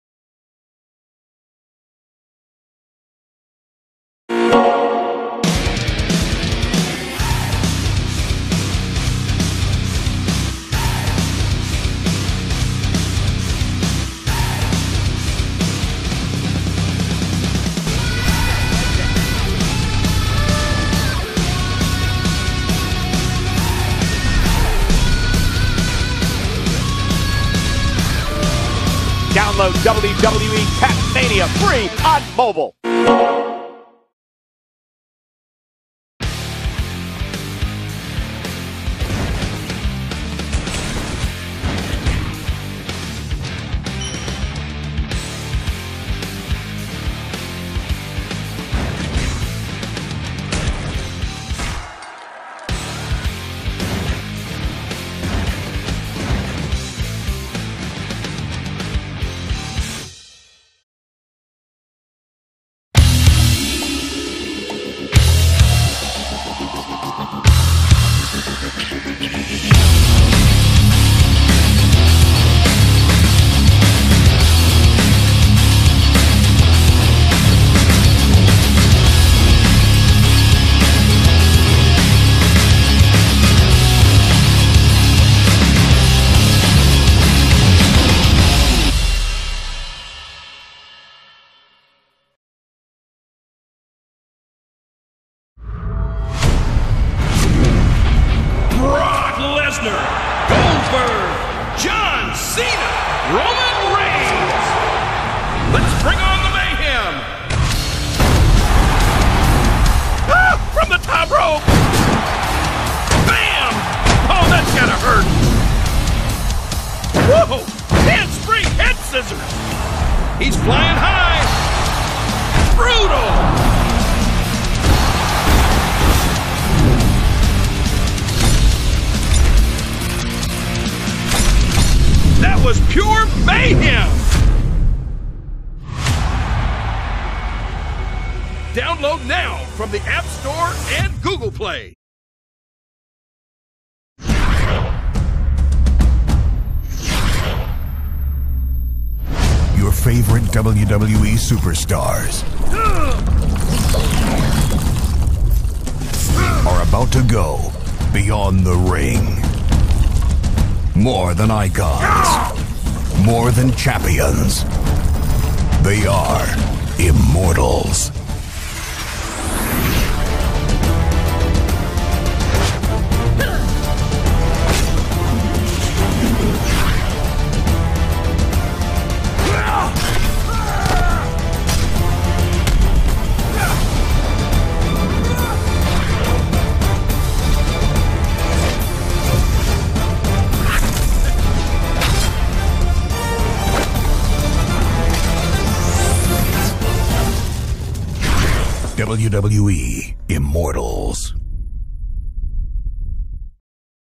"TOP 5 WWE GAMES ON ANDROID"
These are top 5 WWE games on Android and iOS...!!!
Hope you enjoyed this vedio!!!